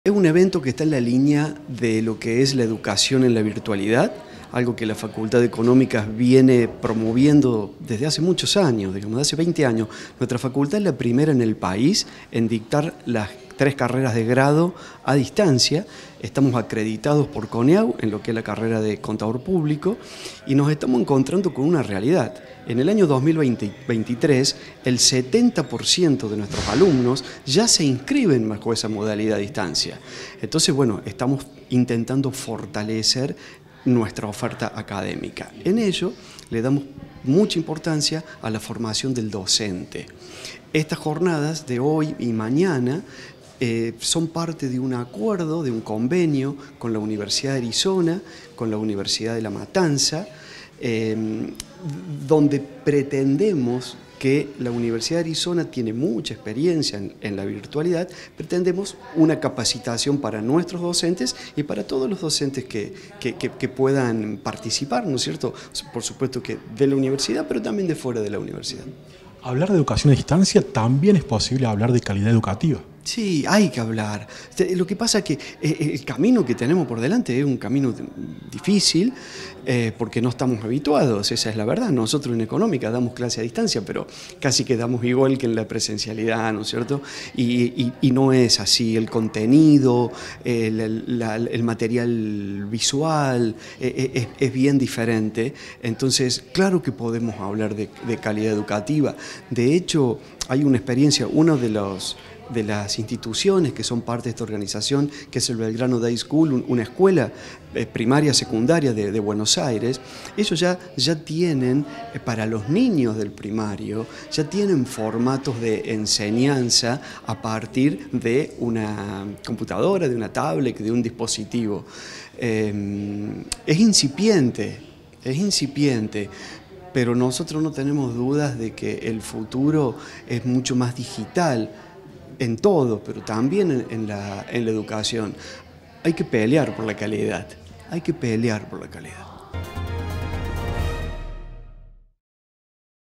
CICLO DE CONFERENCIAS “ENSEÑAR EN LA VIRTUALIDAD” A CARGO DE EXPERTOS DE ARIZONA STATE UNIVERSITY
GUILLERMO MANA – DECANO FACULTAD DE CIENCIAS ECONÓMICAS
UniRio TV
Universidad Nacional de Río Cuarto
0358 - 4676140